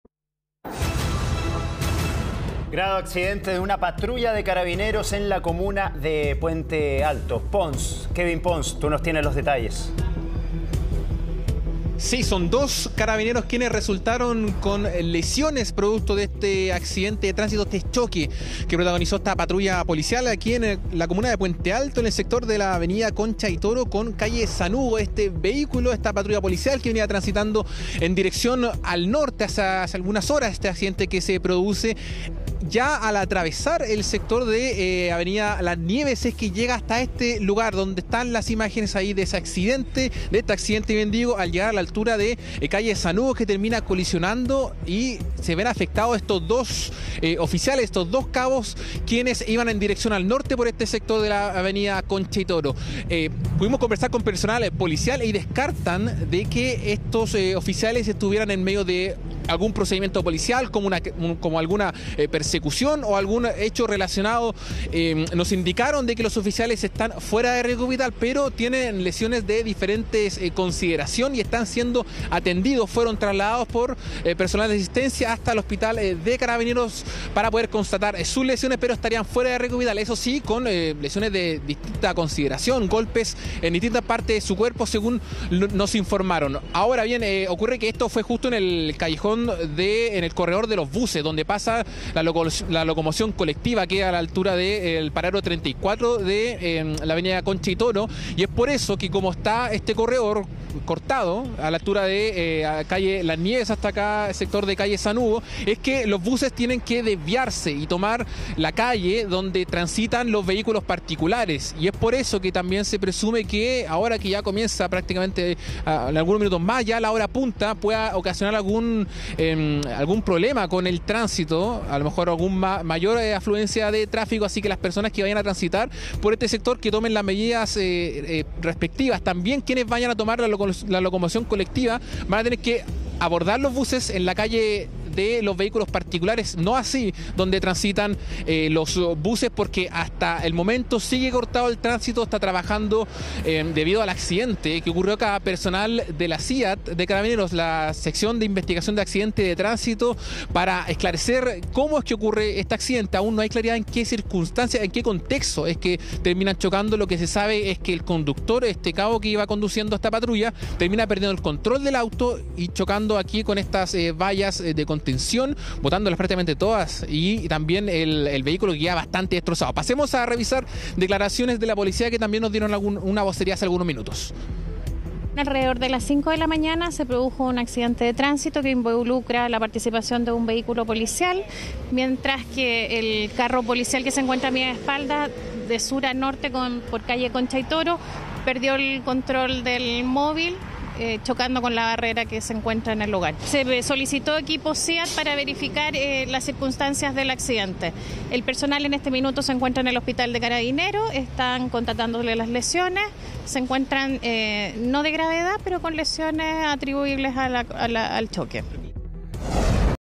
Patrulla de Carabineros terminó DESTRUIDA al incrustarse en reja perimetral en Puente Alto
Desde la institución descartaron que hubiesen estado en un procedimiento policial e informaron que dos funcionarios resultaron heridos producto del choque, pero fuera de riesgo vital.